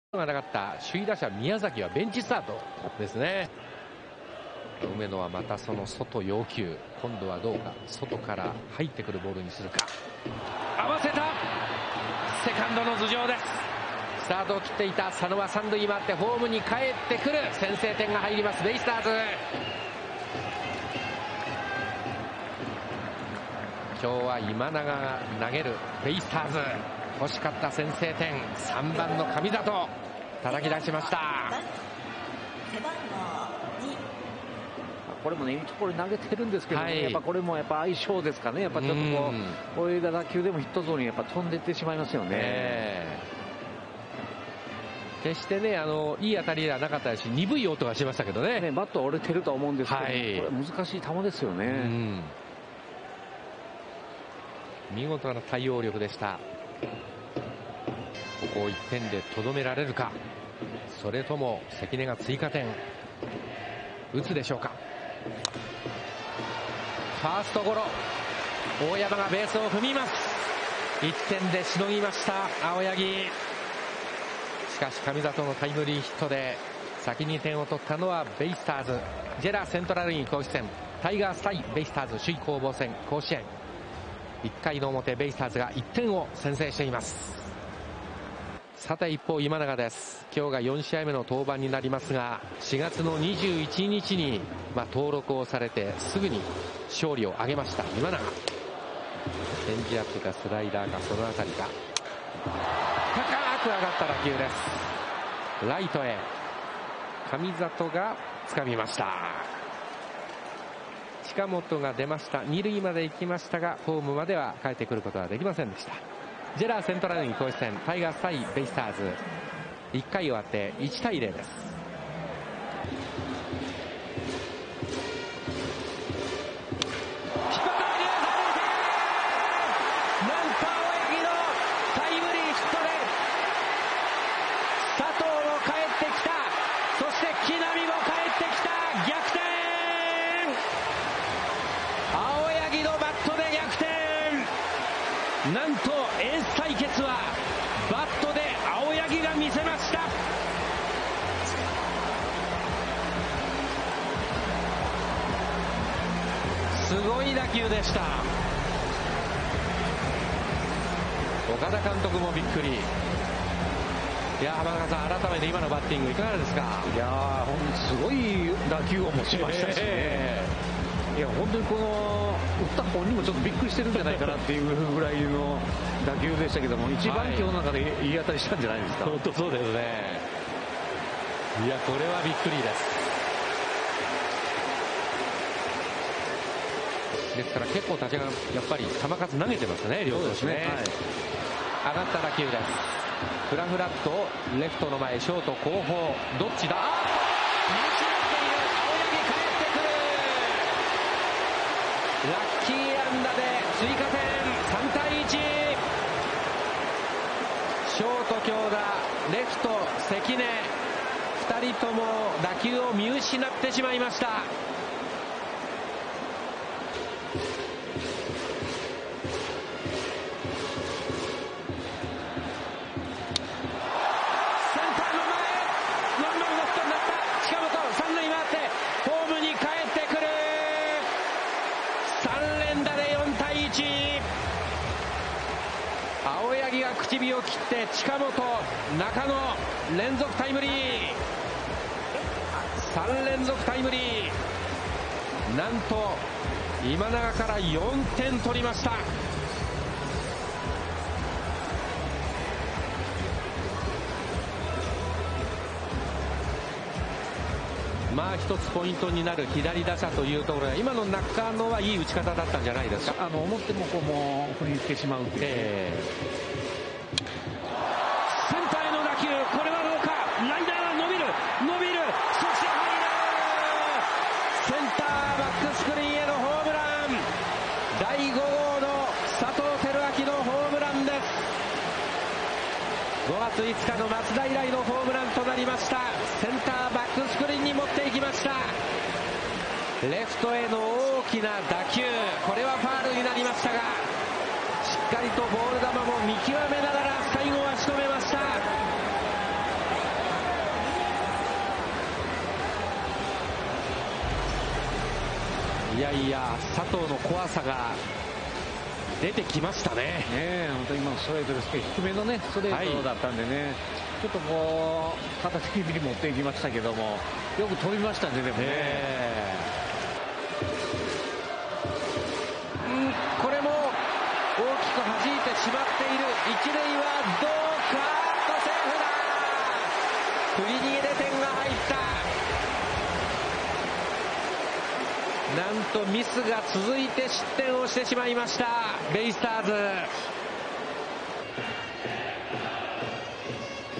🐯阪神タイガース連勝‼️横浜とのゲーム差1に。阪神タイガース
阪神は1点を追う2回裏、青柳、近本、中野の3者連続適時打で4点を挙げ、逆転に成功する。続く3回には、佐藤輝のソロが飛び出し、リードを広げた。投げては、先発・青柳が8回途中3失点の力投で今季2勝目。敗れたDeNAは、先発・今永が4回5失点と試合をつくれなかった。

これで横浜とゲーム差1なりました✨
3タテは厳しいと思いますが何とか勝ち越してほしいです❣️